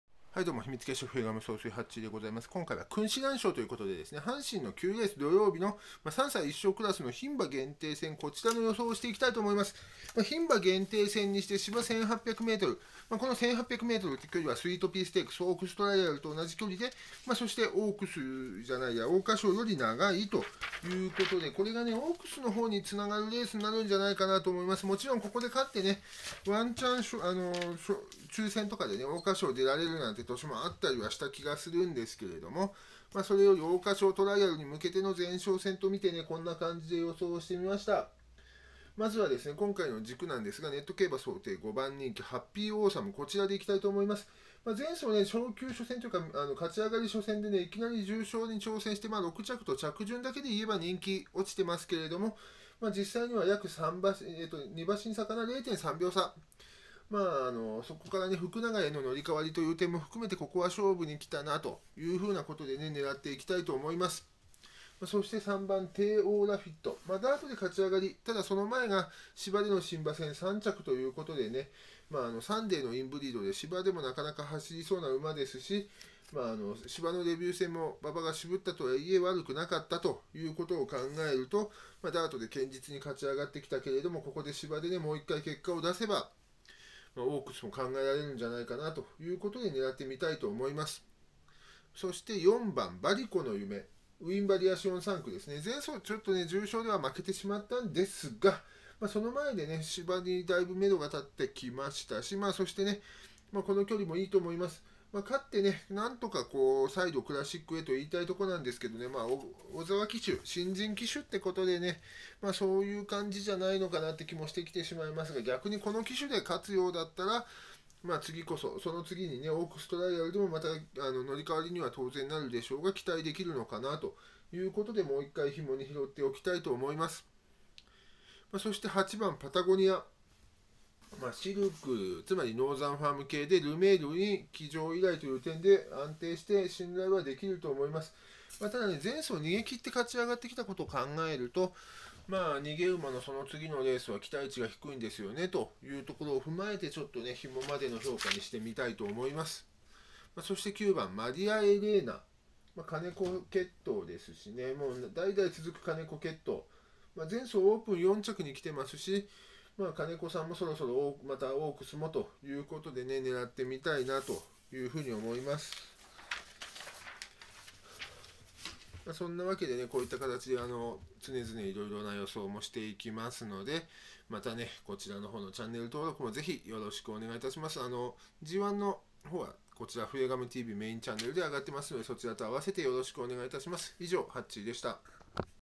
【競馬予想】君子蘭賞(土曜阪神11R 3歳1勝クラス牝)予想
ハッピーオーサム
テーオーラフィット
バリコノユメ
パタゴニア
マリアエレーナ